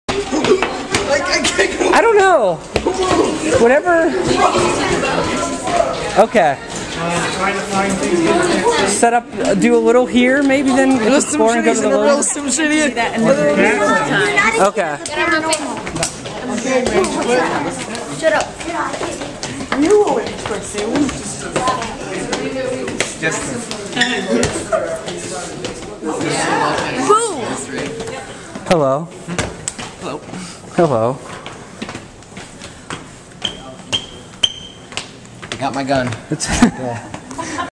Teen After Hours with the Maine Ghost Hunters VIDEO #1
The teens and the Maine Ghost Hunters take the long walk up the stairs to the Portland Room.  We spent about 1.5 hours in the Portland Room investigating all things paranormal.